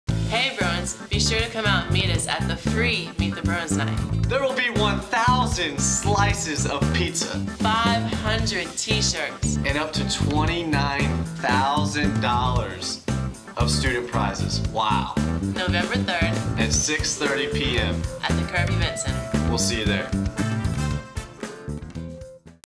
Taylor & Sierra Invite You to Belmont Basketball's Meet the Bruins Night
Belmont Basketball presents Meet the Bruins Night!
Tuesday, November 3rd @ 6:30pm
Curb Event Center
FREE and open to all fans of all ages!!
• Meet & Greet Autograph Sessions before and after the event
• Belmont Men’s Basketball Dunk Contest
• Belmont Women’s Basketball 3-Point Shoot-Out
• Team Scrimmages (see your favorite Belmont players in action & check out their new additions)
• 1,000 Free Slices of Pizza
• 500 Free T-shirts
• Belmont Bruins Photo Booth
• Special Effects Bruins Player Introductions
• Free Belmont Basketball Team Posters, Schedule Magnets & keychain tags
• Chances to Win Belmont Basketball Ticket packages
• Single-Game Tickets and ridiculously low-priced Belmont Basketball Flex Packs will be on sale